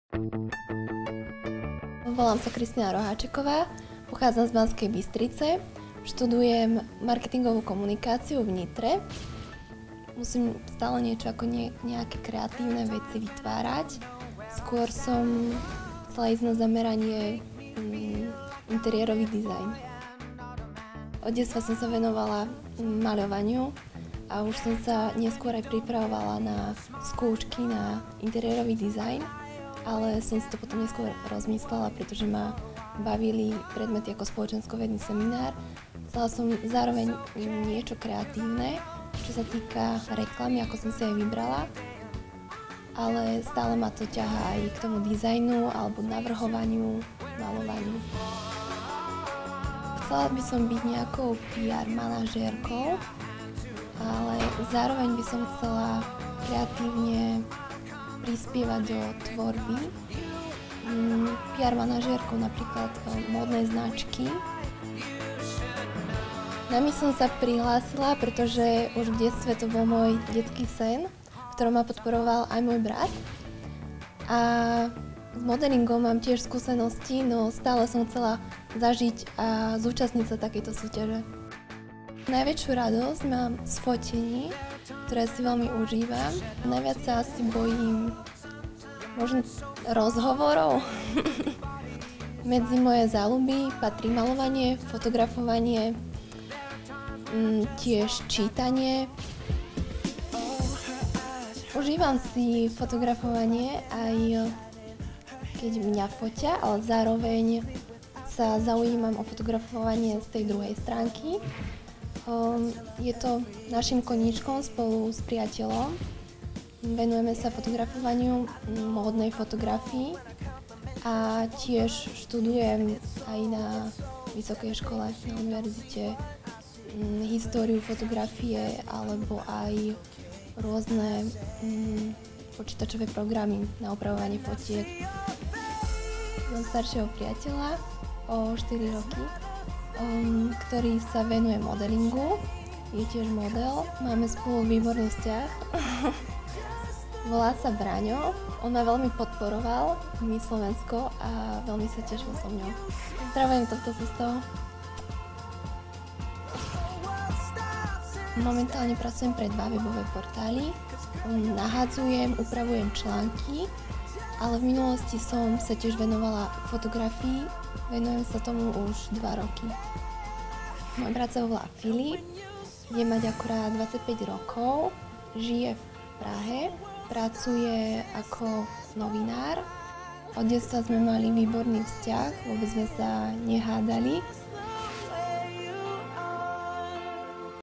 Finalistky MISS SLOVENSKO 2015 na TV JOJ - Kristína Roháčeková
Kristína pochádza z Banskej Bystrice a študuje marketingovú komunikáciu a reklamu v Nitre. Rada kreslí, miluje dizajn a venuje sa aktívne modelingu. V budúcnosti by sa chcela stať PR manažérkou. Spracovala: Alexandra Hinková, TV JOJ